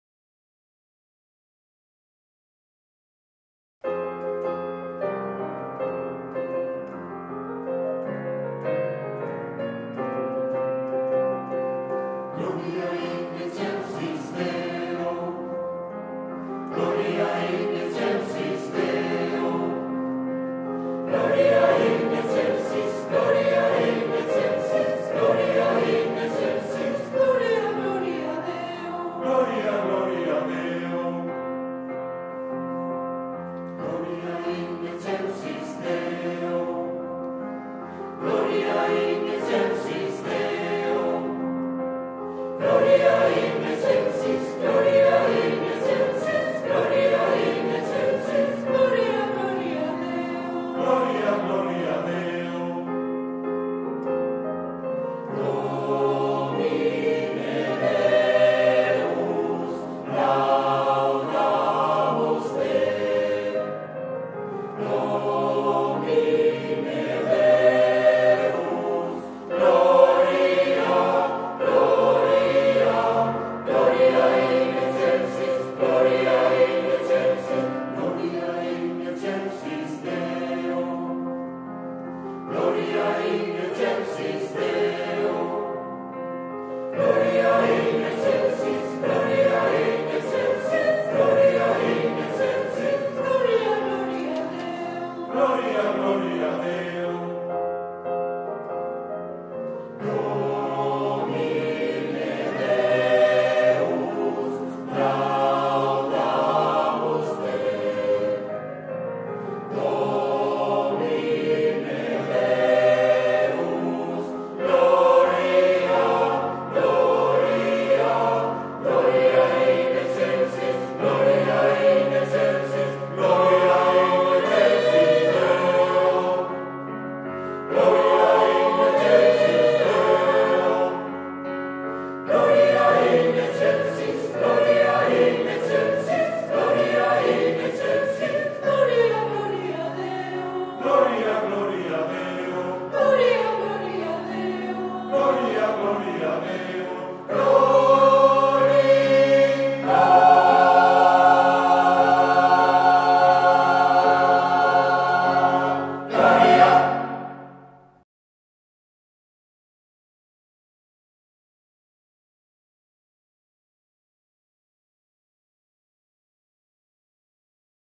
Felicitación de Navidad. Coral Armiz 2022
Felicitación de Navidad de la Coral Armiz coincidiendo con la celebración del 25 Aniversario de su fundación.